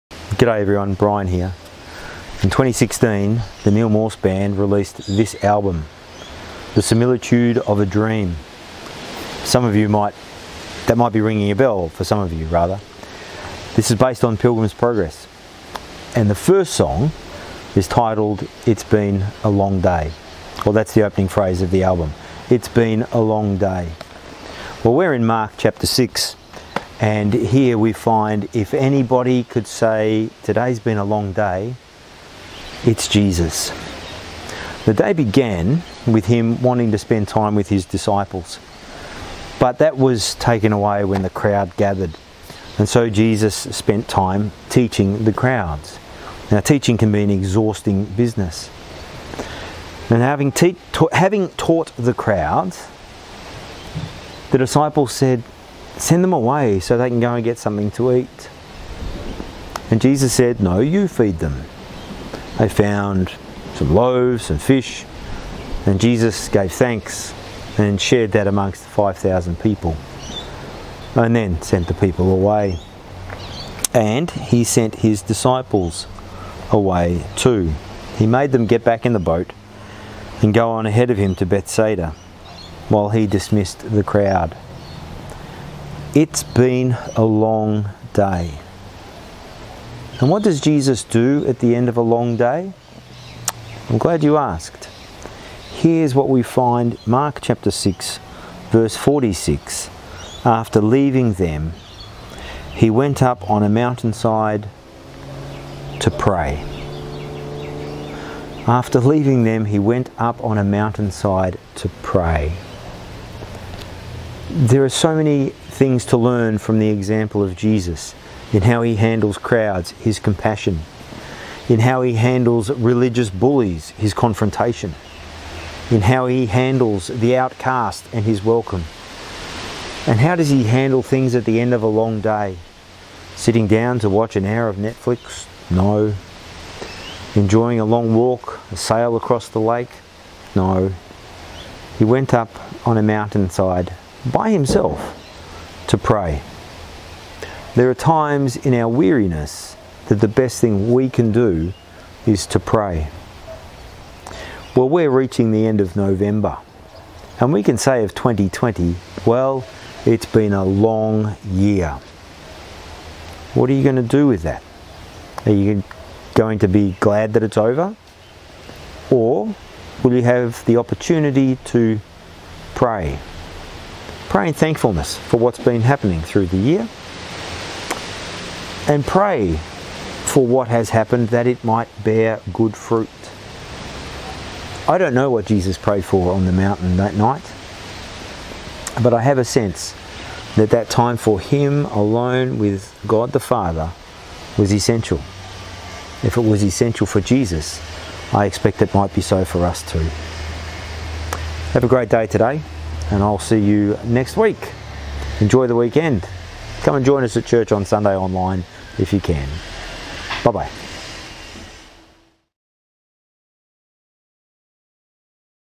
It's been a long day. Mark 6:45-46
We know the feeling of getting to the end of a day and saying to whoever is listening, "It's been a long day." Would you be surprised to know that Jesus understands that feeling? More importantly, would you like to know what Jesus does at the end of the proverbial 'long day'?
Here is your answer.
Mark 6:45-46